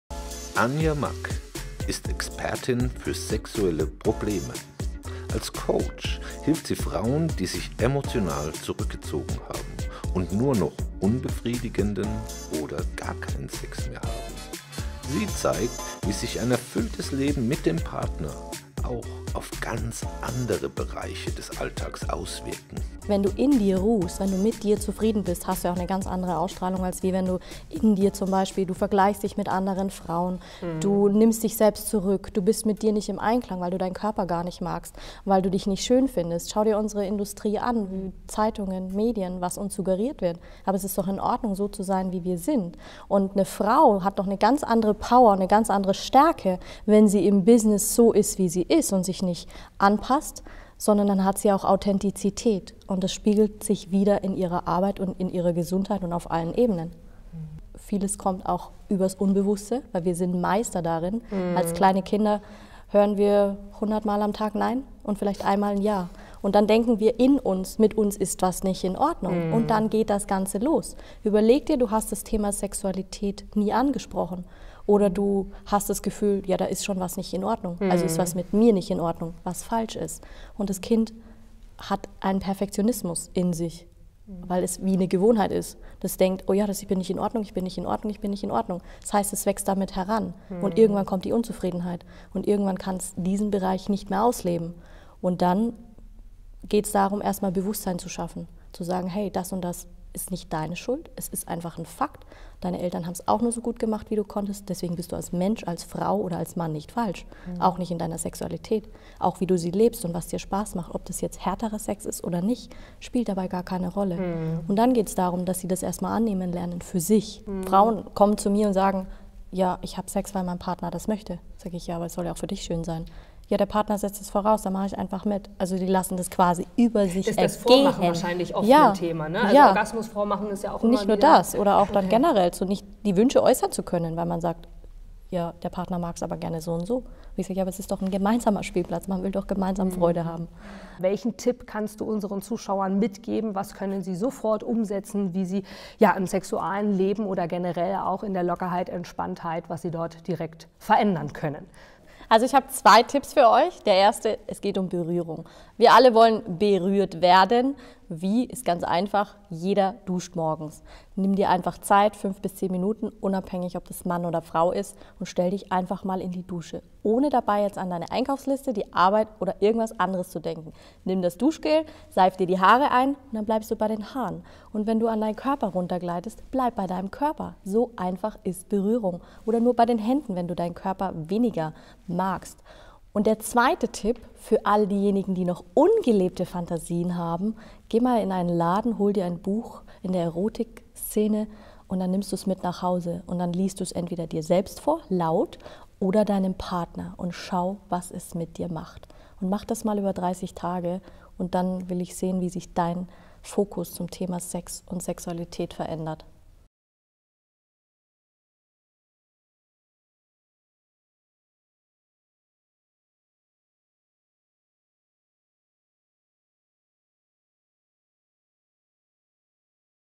Anja Mack ist Sex Coach. Im TV Interview gibt sie Tipps zur verbesserten Sexualität
Anja Mack ist Kooperationspartnerin im Unternehmerbund International. Alle Kontakte finden Sie mit der